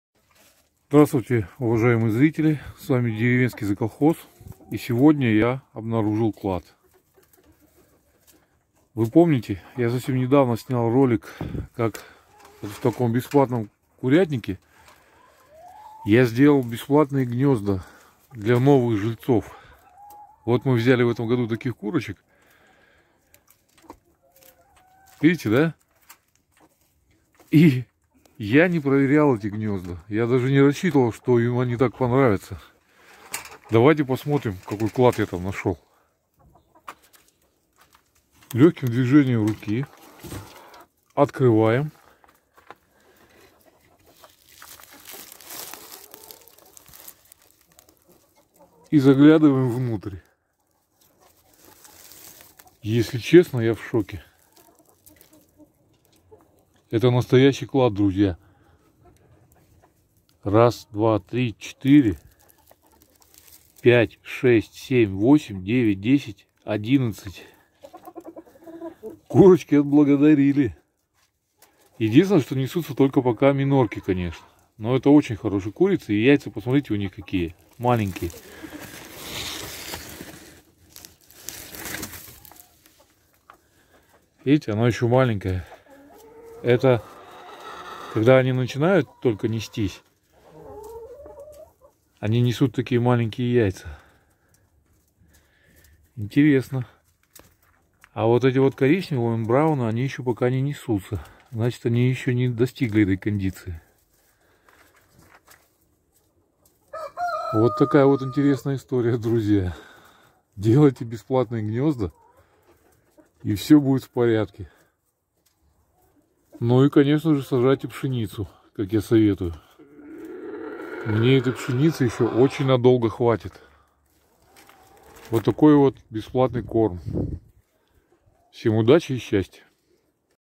Неожиданный подарок судьбы! Клад в курятнике.
Неожиданный подарок судьбы, настоящий клад ждал меня сегодня утром. Не даром есть поговорка Кто рано встает, тому Бог подает.
привязана карта Сбер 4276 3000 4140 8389